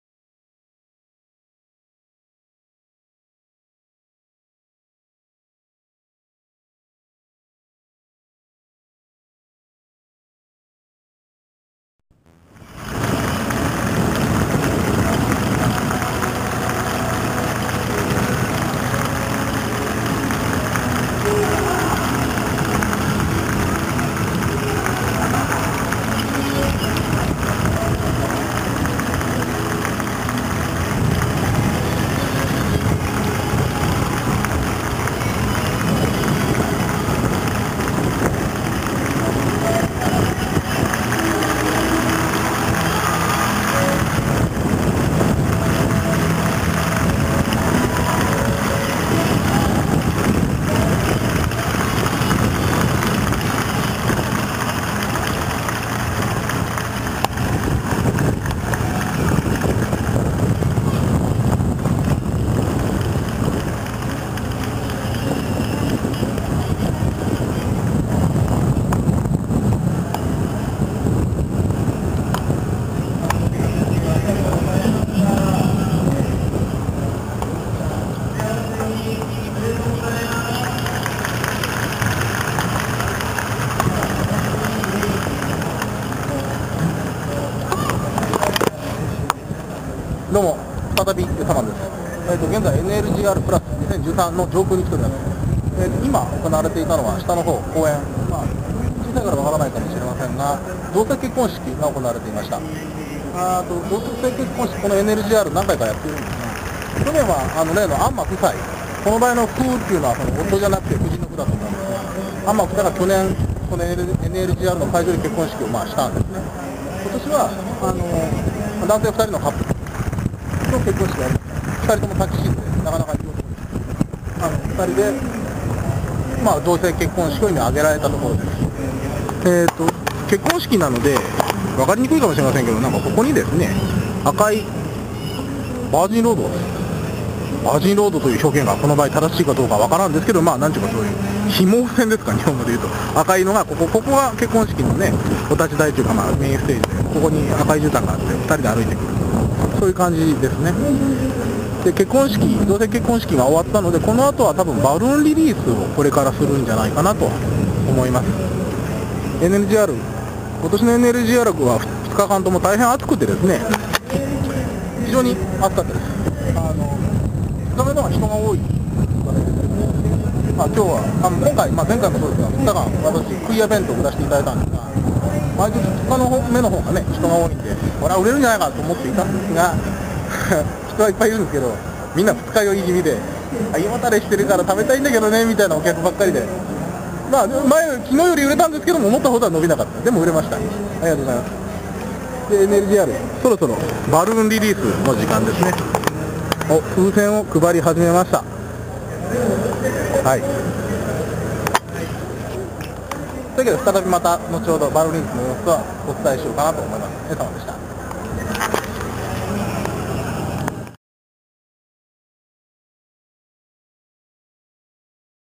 NLGR+2013 二日目A 同性結婚式・上空からのレポート @NLGRplus
●NLGR+2013
日時:6月8日(土)-9(日)終日
場所:池田公園